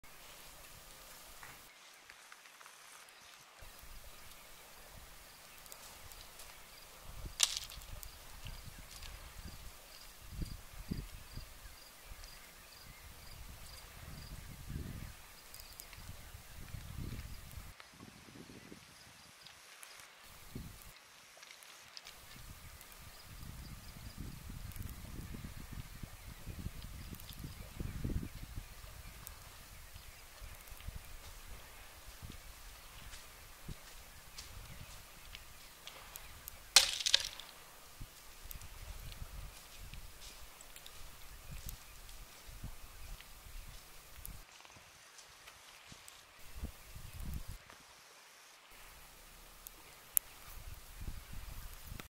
canadian geese
Geese and their goslings cruising the shoreline in Northern Idaho.